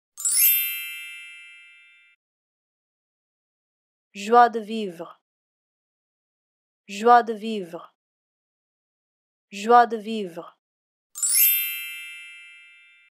How to Pronounce Joie de Vivre
Learn how to pronounce the phrase 'Joie de Vivre' with Unstammer's free pronunciation tutorials. Also read the meaning(s) and usage of Joie de Vivre